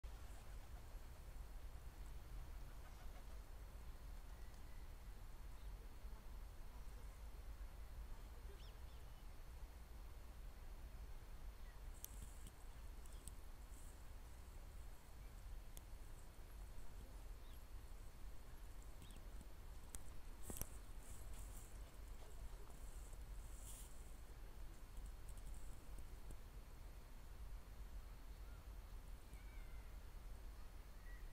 It's amazing
It's amazing and  incredible .The squirrel hides it's food and keep it for future.